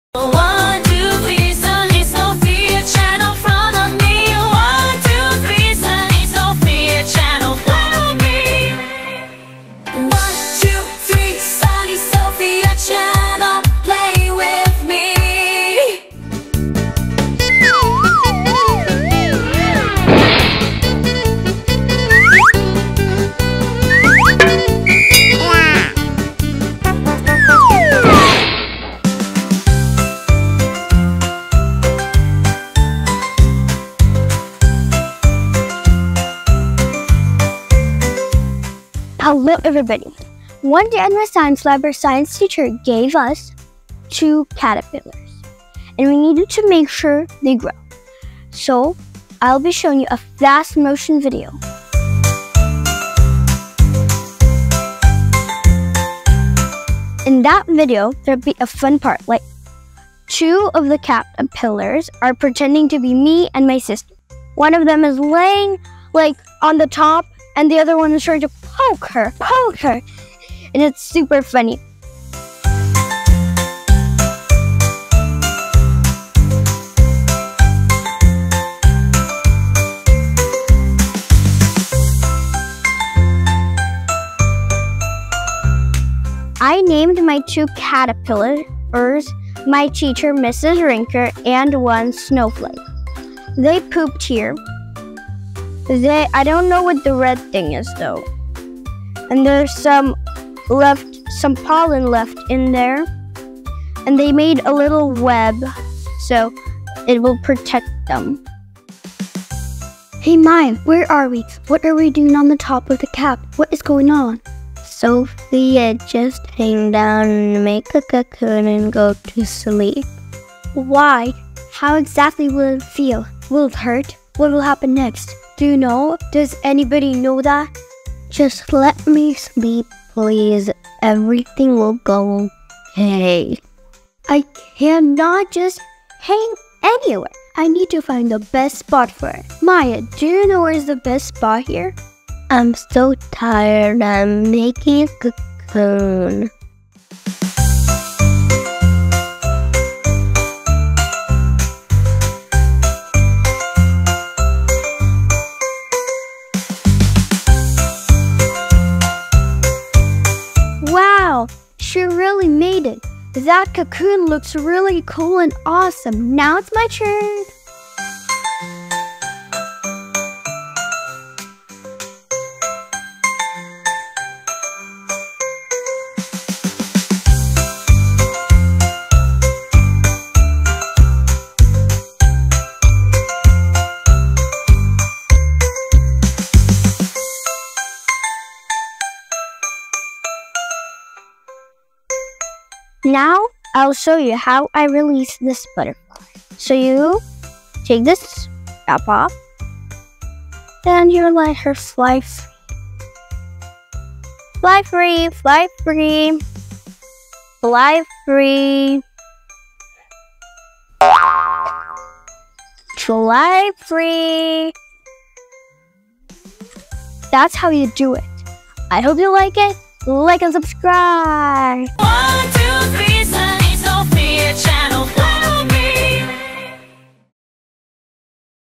Caterpillar turns into a butterfly
Ever wondered what happens when you're given two caterpillars by your science teacher? Join me as I share the magical journey of two tiny caterpillars transforming into beautiful butterflies! 🐛🦋

It all started when my science teacher handed me a plastic cup with two tiny caterpillars and loads of pollen. At first, they were so small, but as they feasted on the pollen, they grew bigger and fatter right before my eyes!

Then, something incredible happened. When the time was right, they climbed to the top of the lid. One of them spun a web-like substance and formed a cozy cocoon. The other caterpillar seemed curious and kept poking its friend until it too decided to make its own cocoon.

Days passed, and I watched with anticipation as the cocoons transformed. Finally, they emerged as beautiful butterflies, ready to spread their wings and explore the world! 🌟 Join me as I release these newfound friends into the wild, witnessing the wonder of nature firsthand!